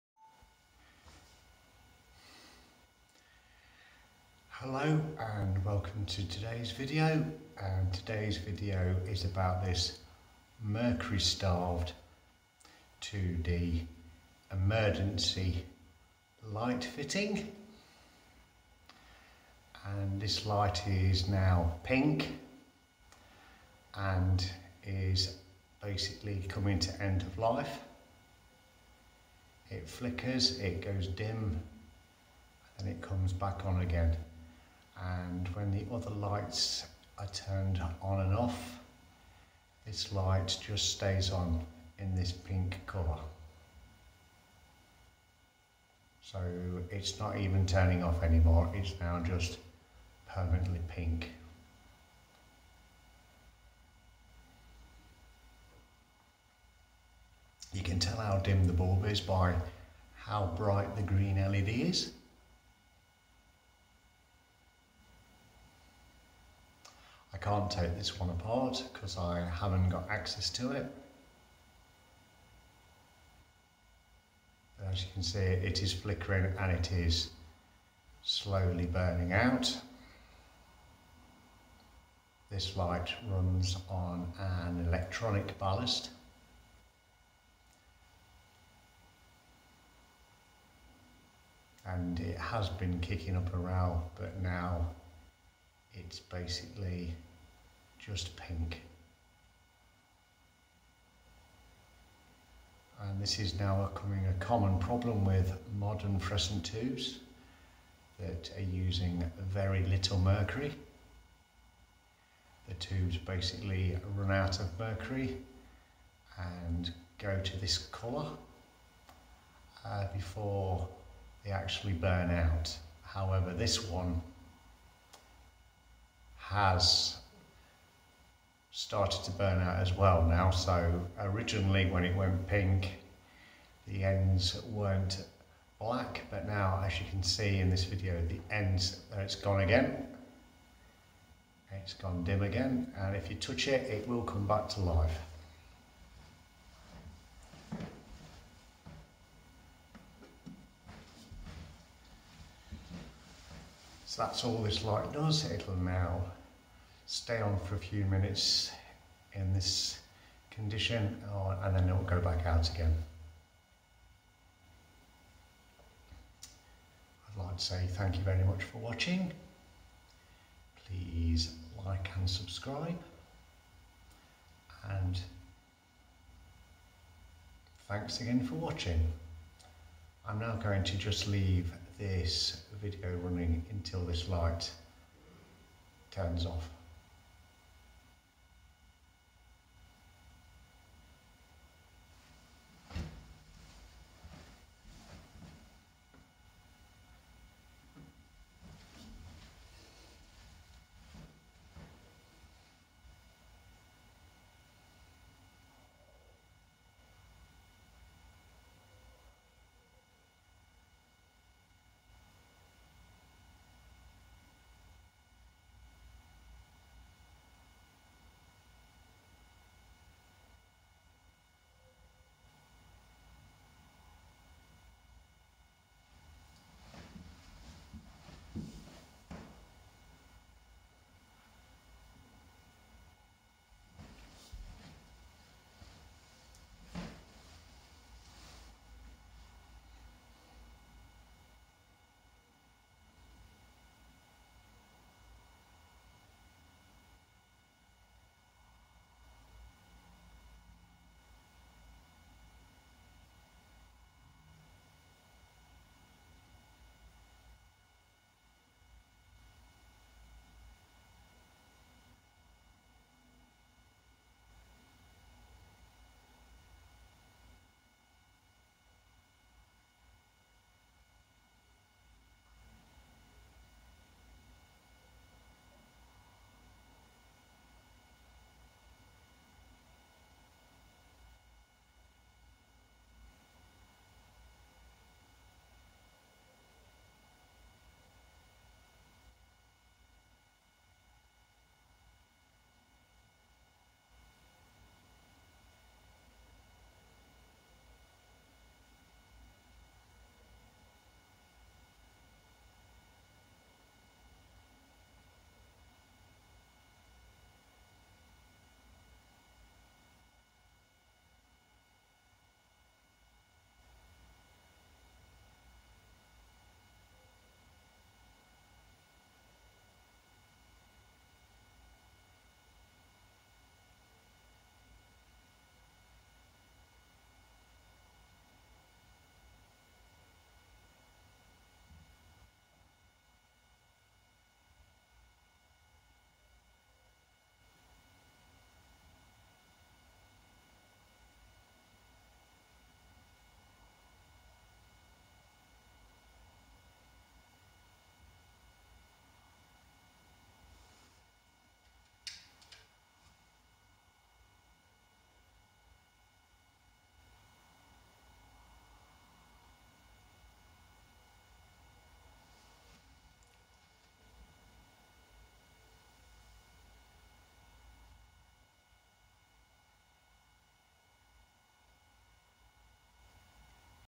EOL 2D Emergency Electronic Fluorescent Light
This Emergency Light with a 2D Fluorescent Tube possibly 28 watt has turned pink. Over the last month the tube has started to get dimmer and getting a pink hue. Today the cathodes have started to burn out. The noise is the video is coming from the light, not sure if its the electronic main ballast for normal lighting or the back up loss of power ballast. What I do know this light should not be on when all the others in the stairwell are turned off. Green LED shows mains is on!!
Fluorescent Tubes that start of white go dim and turn pink are normally Mercury Starved, the pink colour is neon that helps fluorescents start up.